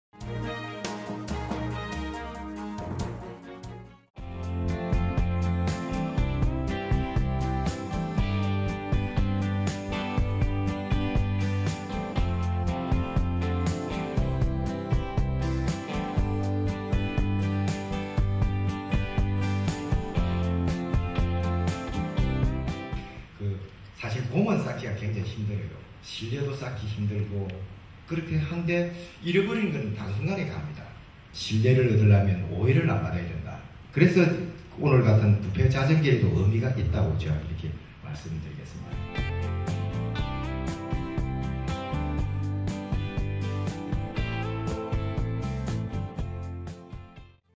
[조달청장 현장뉴스] 2014년 조달청 청렴실천 직원 워크숍
지난 9월 26일 ‘부정부패 척결을 위한 의식개혁 자정운동’의 일환으로 청렴실천 워크숍이 있었습니다. 이번 워크숍은 범정부적으로 추진하고 있는 부패척결 자정운동 확산을 위해 간부 청렴실천 워크숍에 이은 전 직원이 참석한 워크숍입니다.

금품·향응수수 금지, 알선·청탁 안하기 등 청렴서약서 실천 결의대회를 통하여 부정부패척결에 대한 실천의지를 다졌습니다.

신뢰는 청렴성이 뒷받침 되어야 한다는 김상규 조달청장... 그 현장을 동영상으로 만나 보시죠~